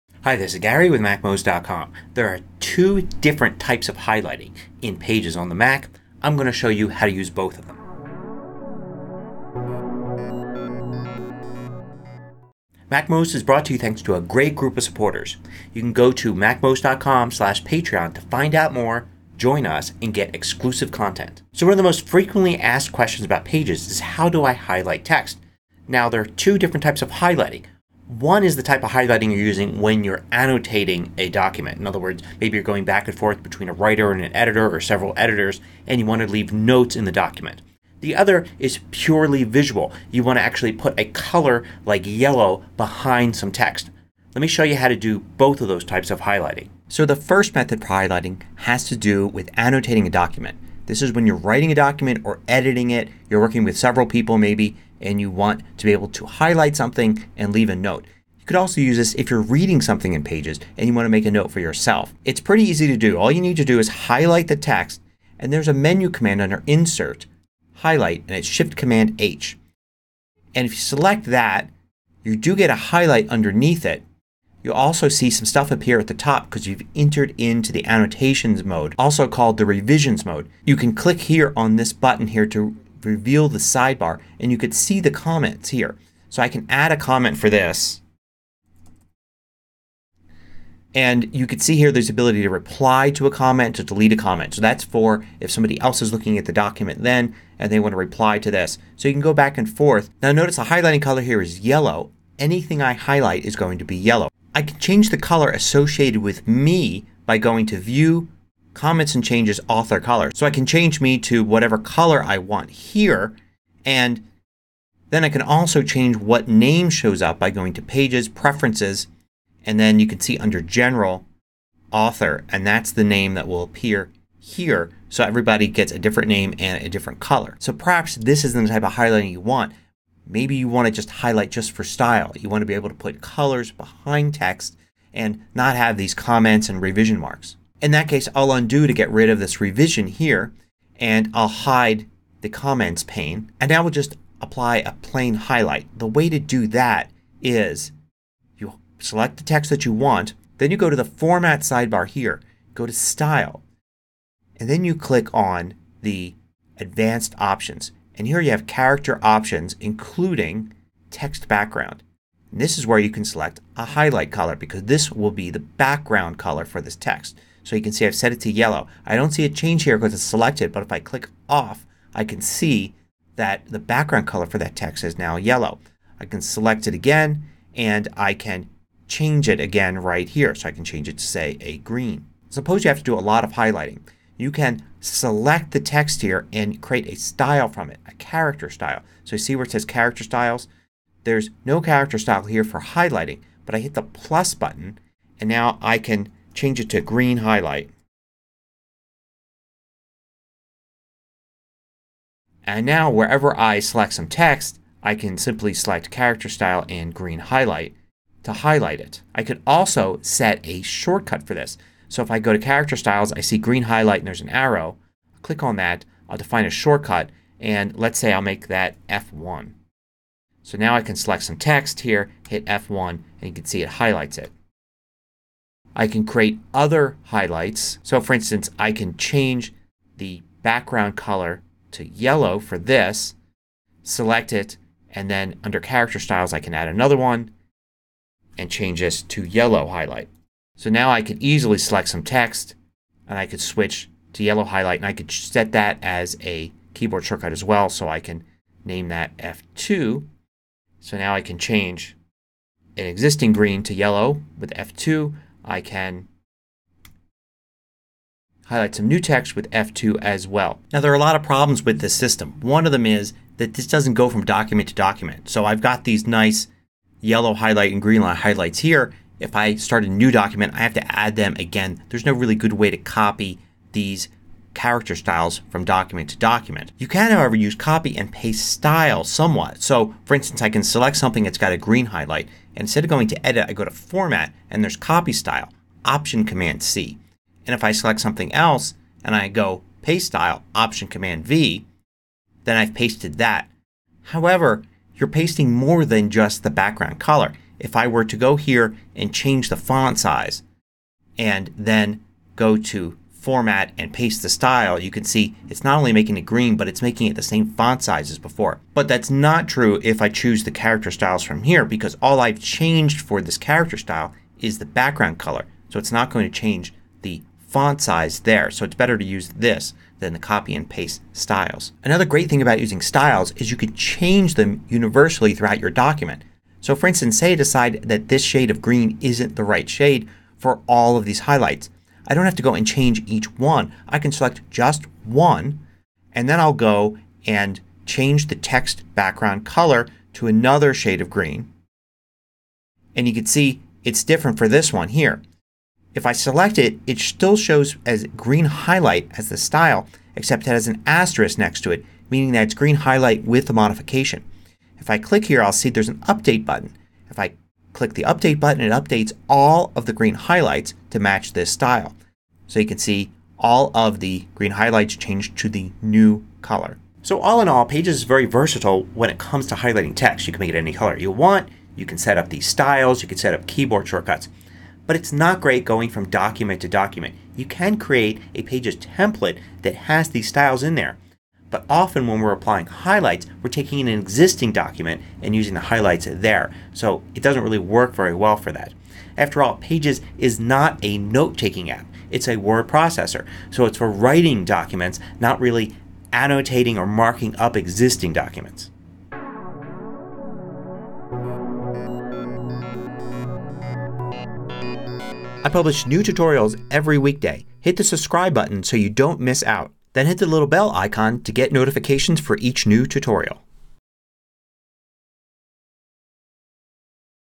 How To Highlight Text In Mac Pages
There are two ways to highlight text in Pages on the Mac. The first has to do with editing and revisions, and is usually used when collaborating on a document with others. But you can also highlight text purely as a style, placing a solid color behind characters. To make things easier, you can create character styles for different text highlight colors and even assign keyboard shortcuts to them.